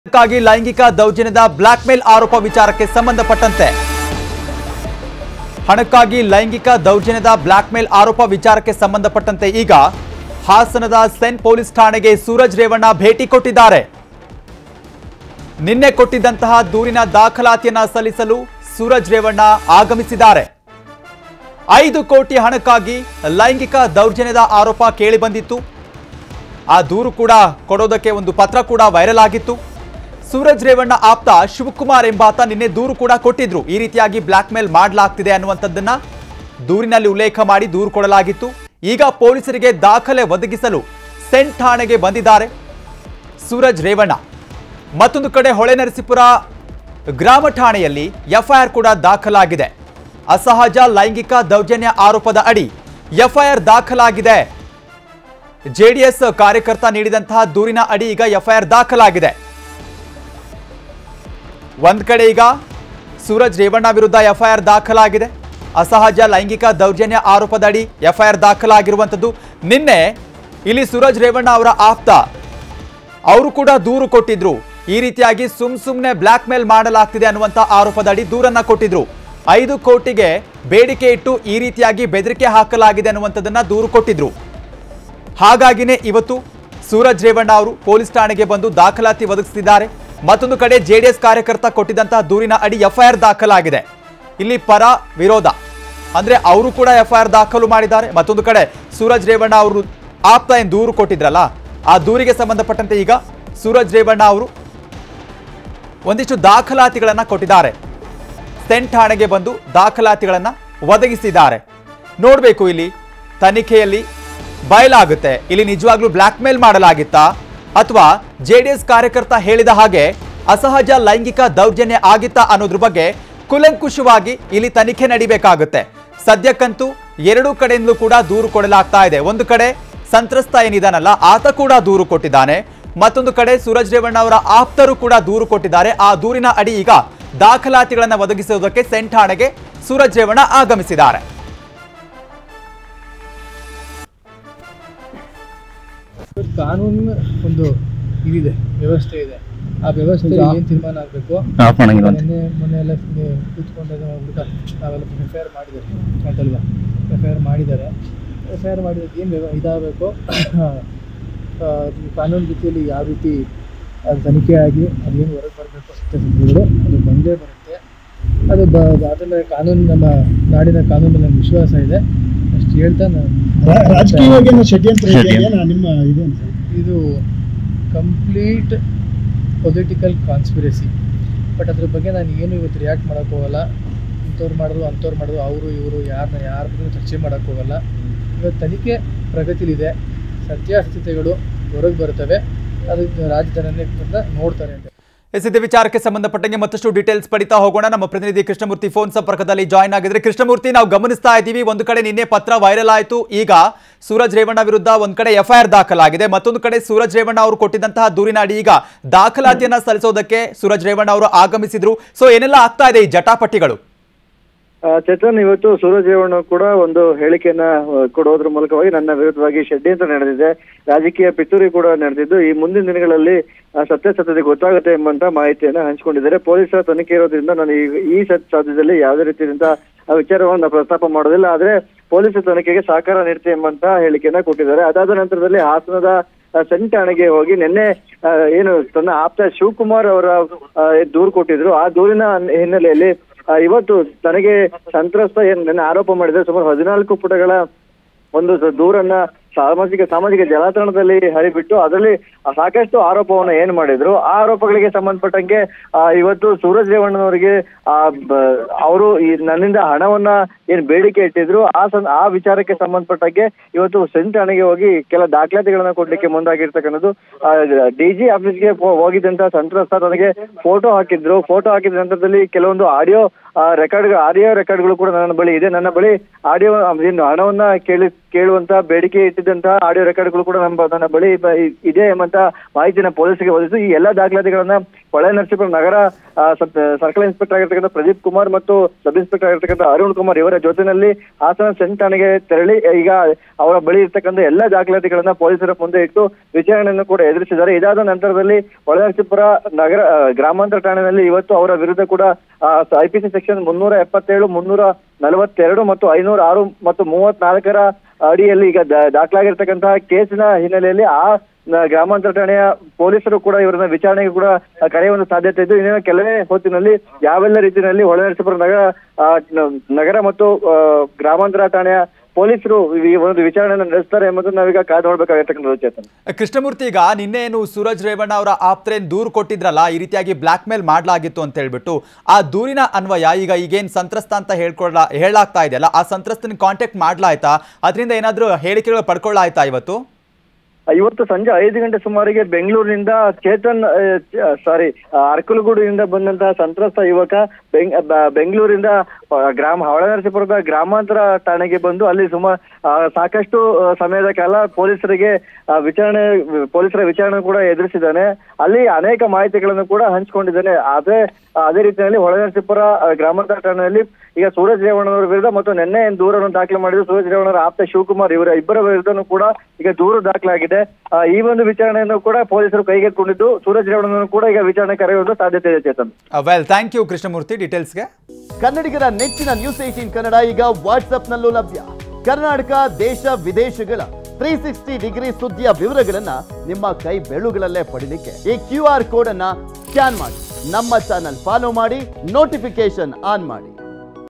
Suraj Revanna Visit Hassan Police Station | ₹5 ಕೋಟಿ ಹಣಕ್ಕಾಗಿ ಲೈಂಗಿಕ ದೌರ್ಜನ್ಯದ ಆರೋಪ | HD Revanna
News18 Kannada Live | ನ್ಯೂಸ್ 18 ಕನ್ನಡ ಲೈವ್
Karnataka | ಕರ್ನಾಟಕ
Kannada | ಕನ್ನಡ